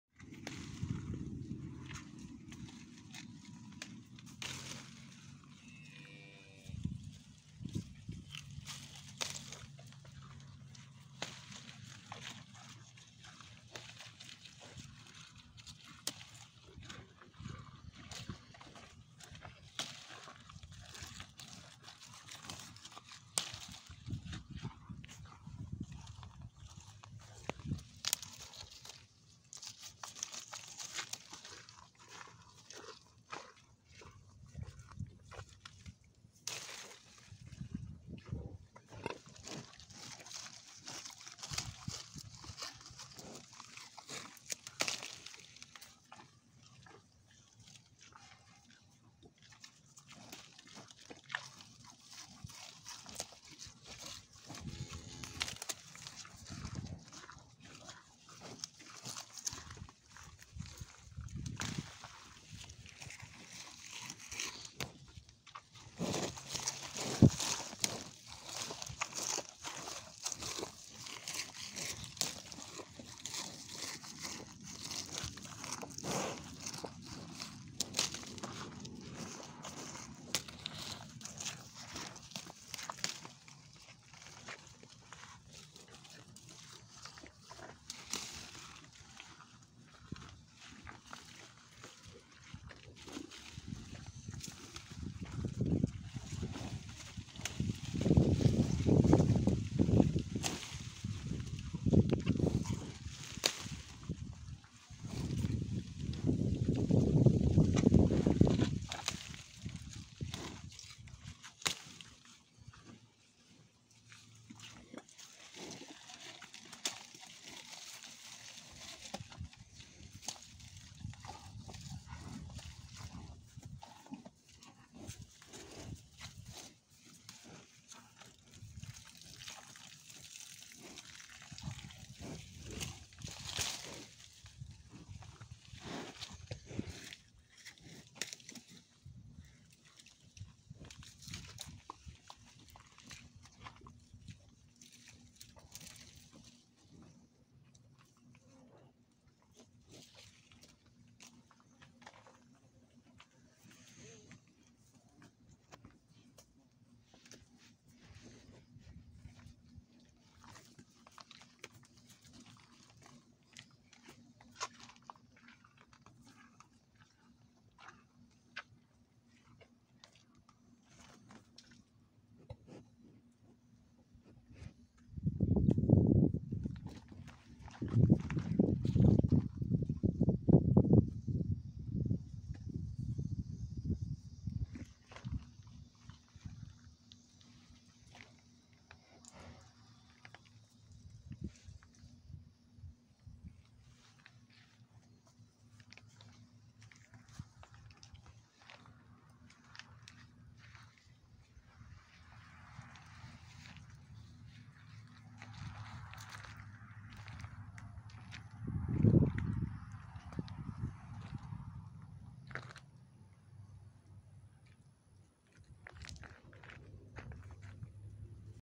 baby cow loves trees
this is a small cow and a few others joined they really enjoy tree leaves